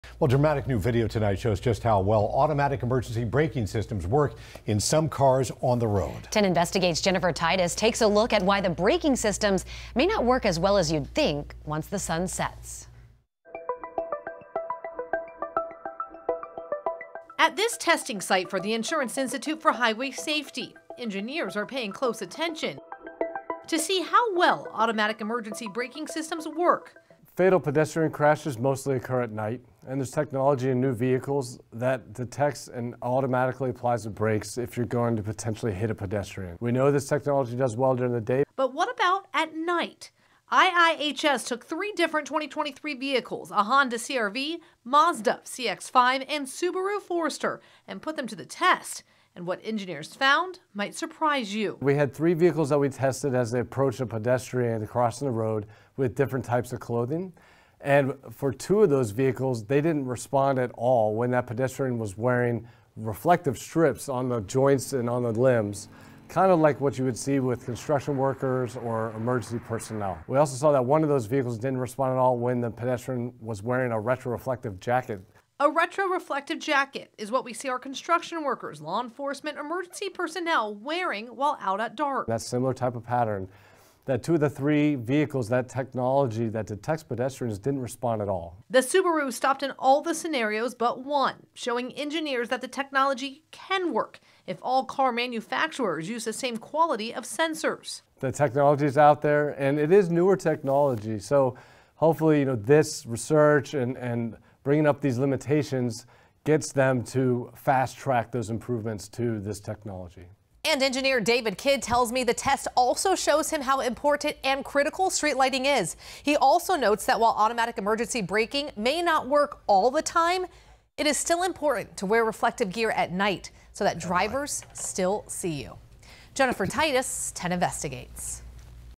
Study tests how well cars' automatic emergency brakes work at night
At a testing site for the Insurance Institute for Highway Safety (IIHS), engineers are paying close attention to see how well Automatic Emergency Braking (AEB) systems work when it comes to spotting someone trying to cross the street.

“Fatal pedestrian crashes mostly occur at night, and there's technology in new vehicles that detects and automatically applies and brakes if you're going to potentially hit a pedestrian. We know this technology does well during the day,” David Kidd with the IIHS said.

But what about at night? IIHS tested three different 2023 cars: a Honda CRV, a Mazda CX5 and a Subaru Forester.

“We had three vehicles that we tested as they approached a pedestrian across the road with different types of clothing, and for two of those vehicles, they didn't respond at all when that pedestrian was wearing reflective strips on the joints and on the limbs, kind of like what you would see with construction workers or emergency personnel,” Kidd said. “We also saw that one of those vehicles didn't respond at all when the pedestrian was wearing a retro-reflective jacket.”

A retro-reflective jacket is what we see our construction workers, law enforcement and emergency personnel wearing while out at dark.

“That similar type of pattern that two of the three vehicles that technology that detects pedestrians, didn't respond at all,” Kidd added.